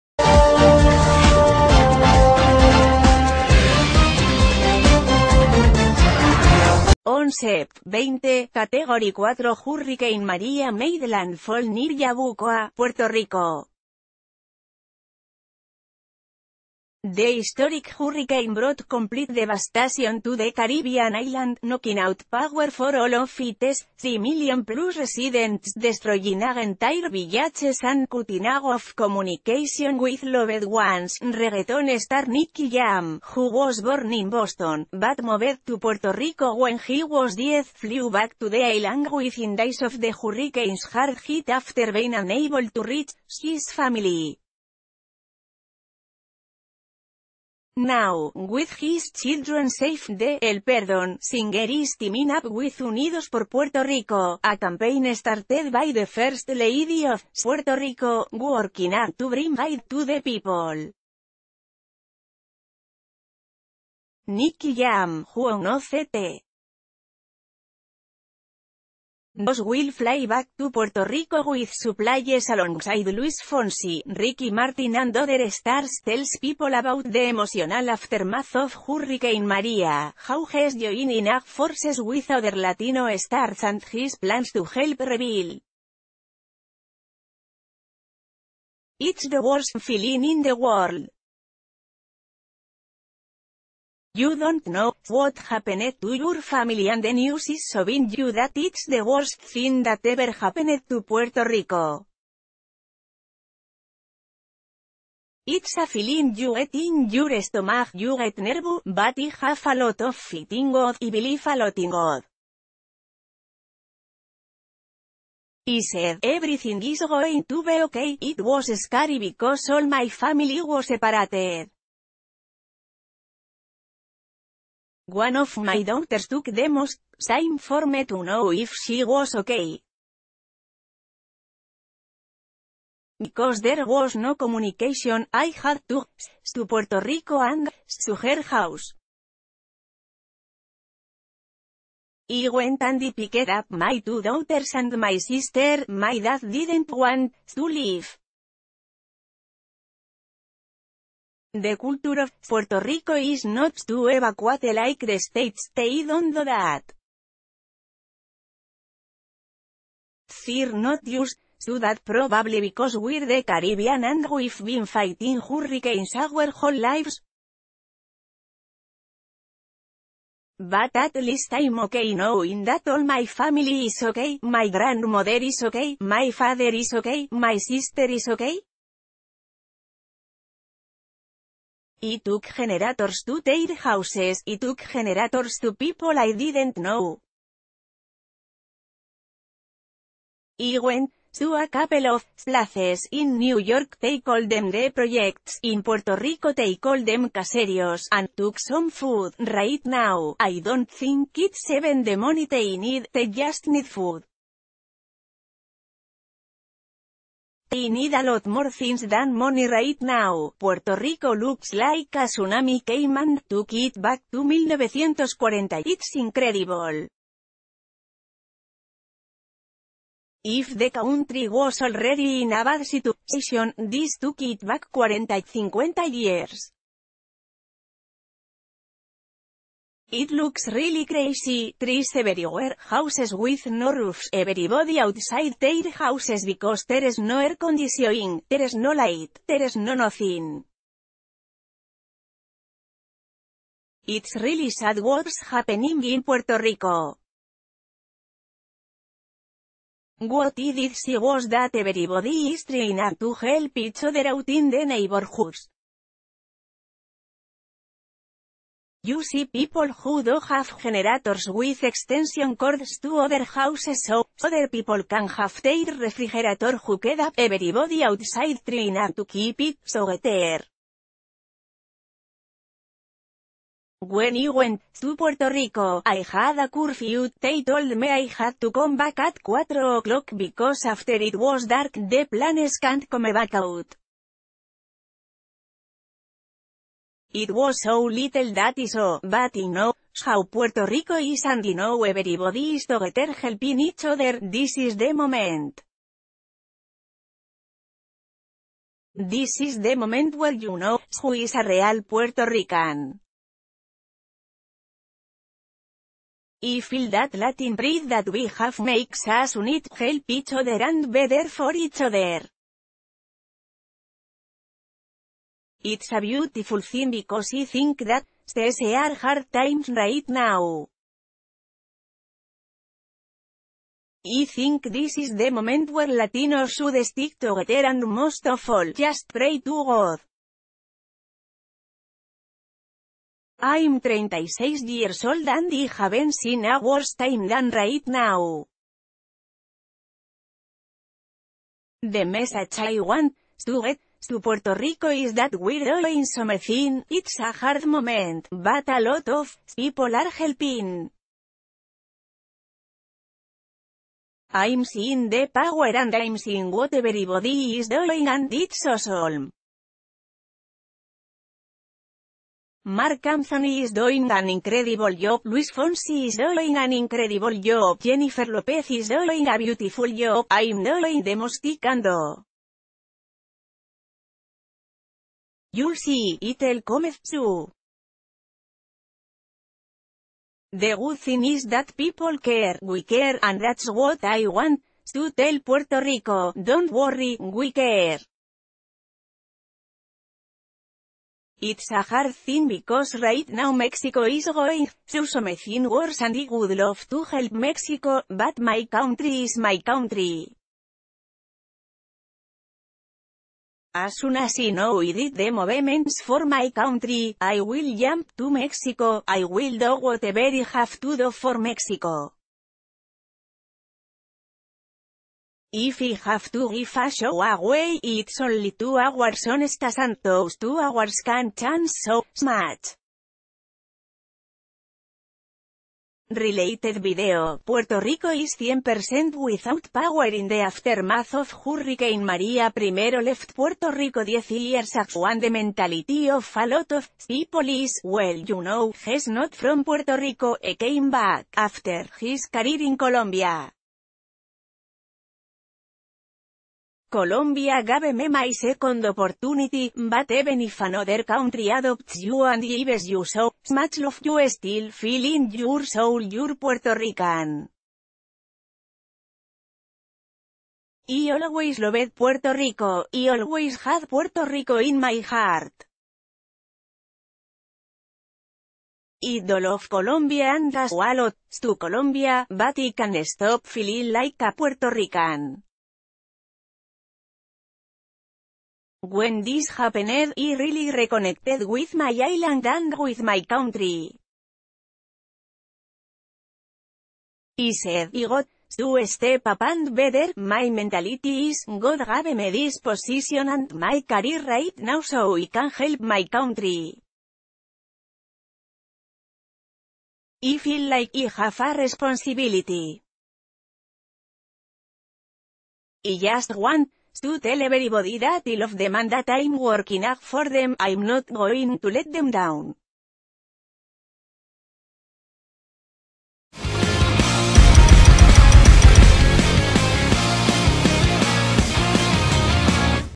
Nicky Jam Flew to Puerto Rico After Hurricane Maria When He Didn't Hear from His Daughter: 'It's ...
Nicky Jam Flew to Puerto Rico After Hurricane Maria When He Didn't Hear from His Daughter: 'It's the Worst Feeling'

On Sept. 20, category 4 hurricane Maria made landfall near Yabucoa, Puerto Rico. The historic hurricane brought complete devastation to the Caribbean island, knocking out power for all of its three million plus residents, destroying entire villages and cutting off communication with loved ones.  Reg...